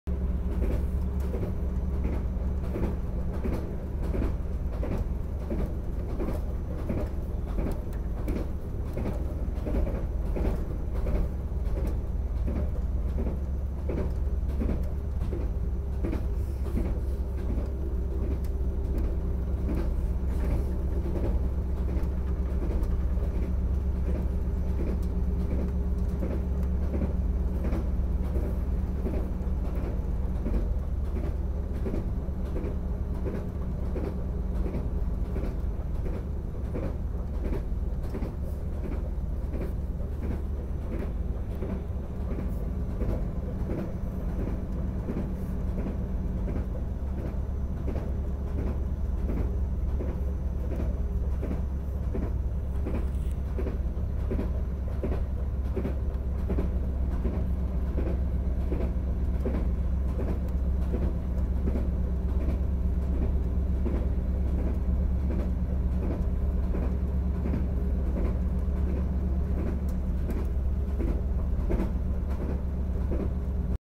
Med Norrtåg i norrländskt höstlandskap. (Lycksele-Hällnäs)
Mellan Lycksele och Hällnäs med Norrtåg Y31 26/9 2021.